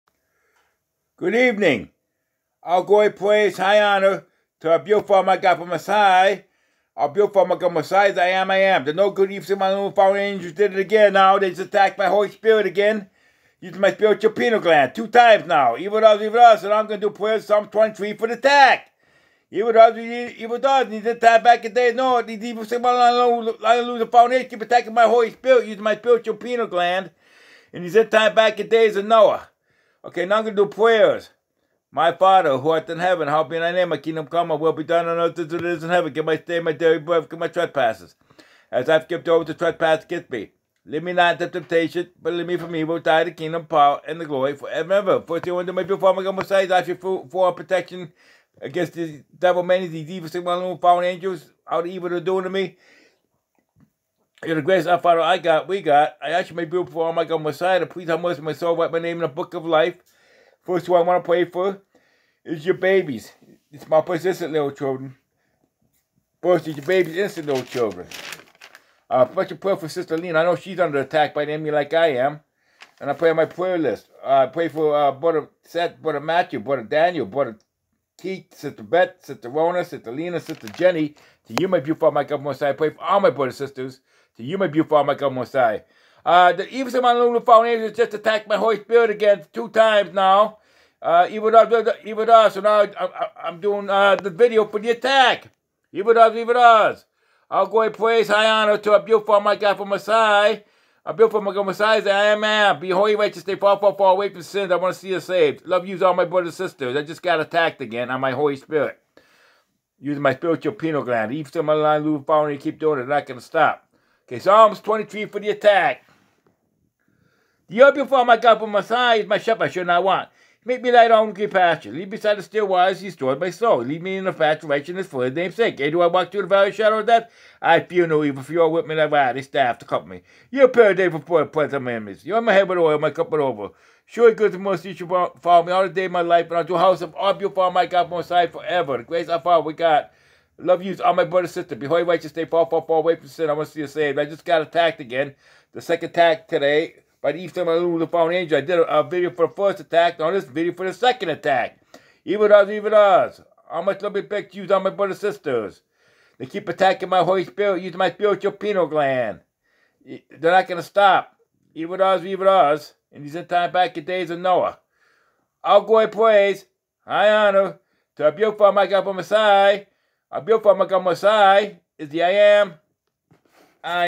thenogood evil sickos demonic lieng loser fallen angels diditagain now 2 timesattacked myholy spirit
evil does what evil does... the sickos demonic lieng loser fallen angels just attacked my holy spirit again its march 2nd 1053am  i dont care all these humans  they use and all these sickos demonic lieng loser fallen angels is going into the pit lake fire   by our  beautiful Heavenly Father Almighty God The Most High they see all the evil that is being done to me the sickos demonic lieng loser fallen angels angels use all the humans they got on us  to do us evil to do us harm and to mess with us ...liars the sickos demonic lieng loser fallen angels used dews( direct energy microwave frequency weapons again ) in texas humans deceived by evil